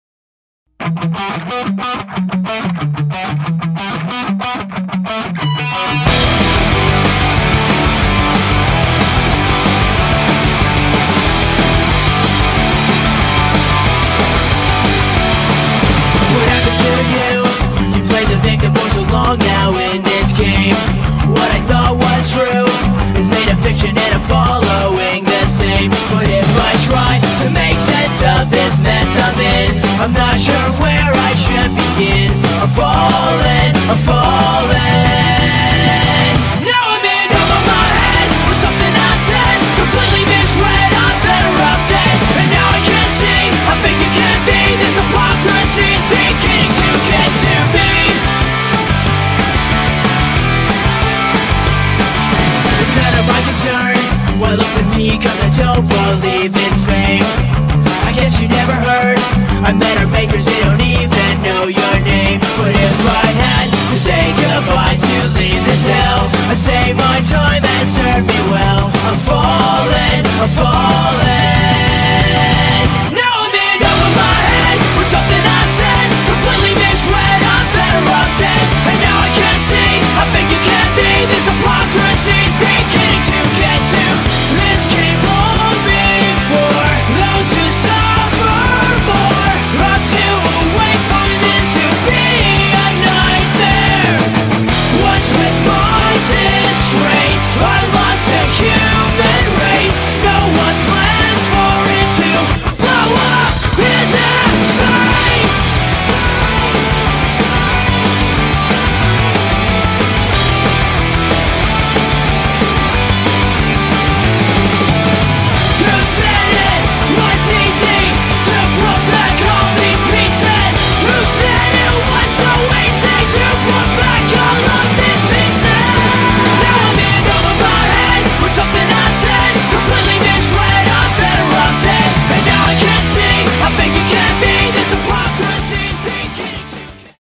Skating
skate video i made for my lil bro and his friend